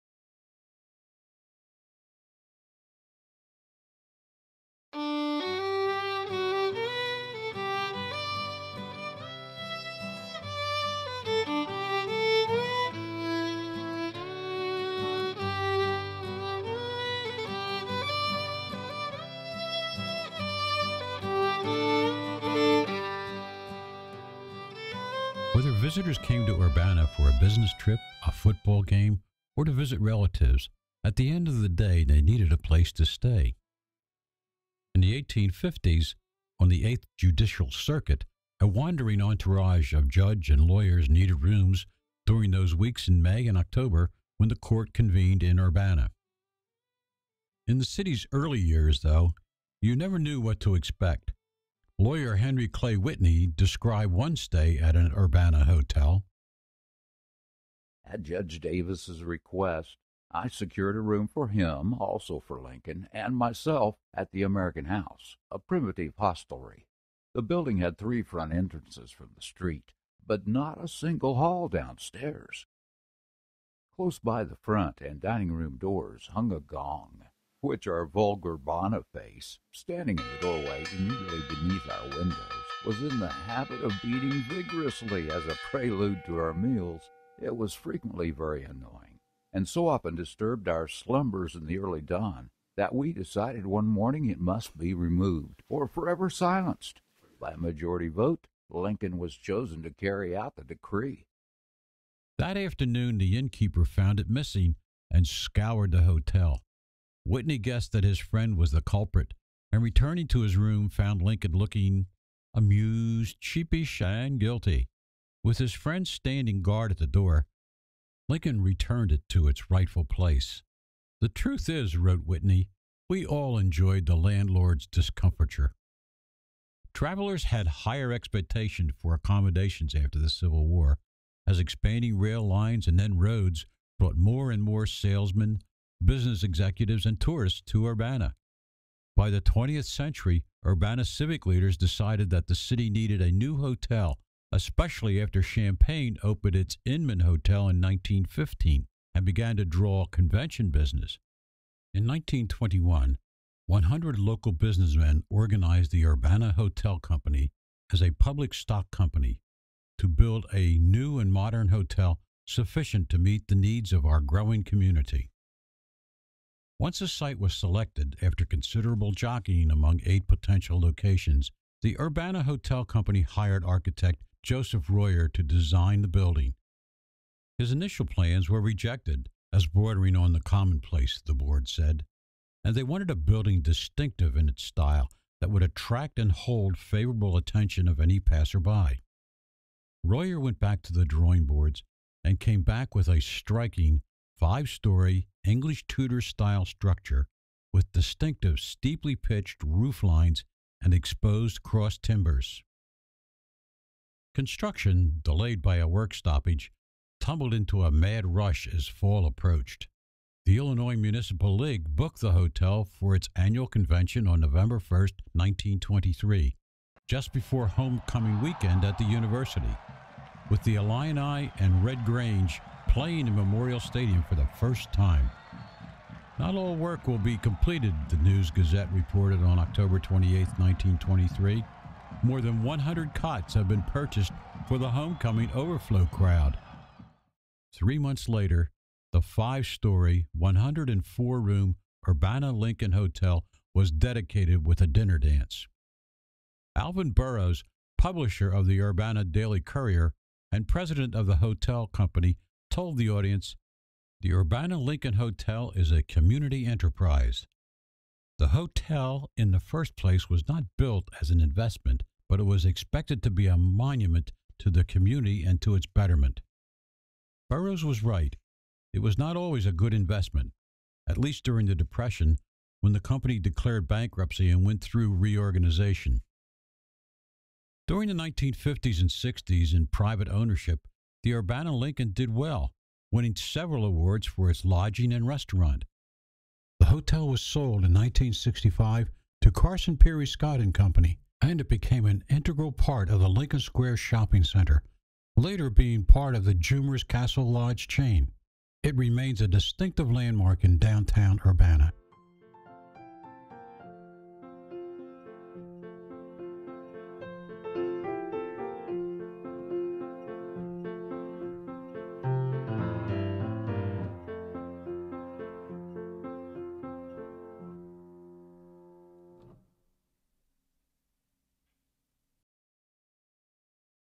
Urbana Lincoln Hotel
Joseph Royer designed this striking five-story English Tudor style structure with distinctive steeply-pitched rooflines and exposed cross-timbers. It opened to the public on November 1, 1923 — just before Homecoming Weekend at the University, with the Illini — and Red Grange — playing in Memorial Stadium for the first time.